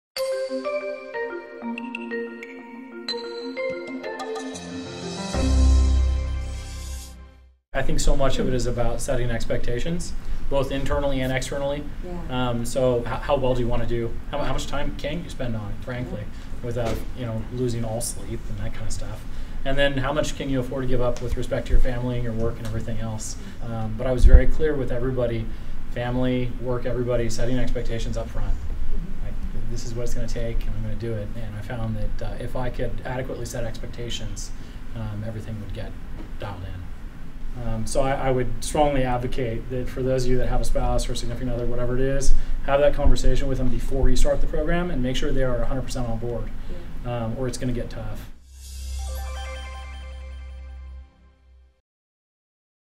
Oregon Executive MBA - Setting Expectations at Home
Setting expectations with your loved ones at home is a key element for your success in the University of Oregon's Portland-based executive MBA program.